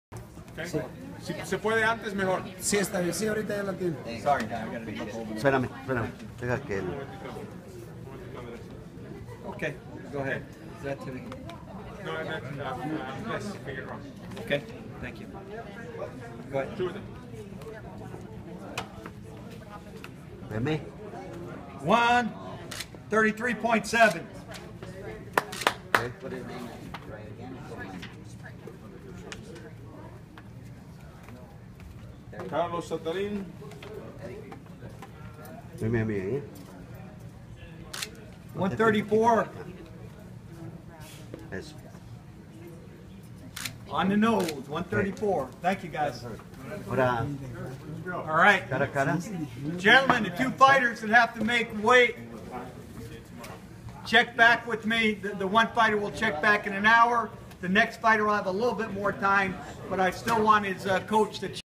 Fabian Figueroa Weigh in - Pesaje - 9-25-14 vs Carlos Zatarain
The weigh in of Fabian Figueroa who faces Carlos Zatarain on 9-26-14.  El Pesaje de Fabian Figueroa quien pelea contra Carlos Zatarain 9-26-14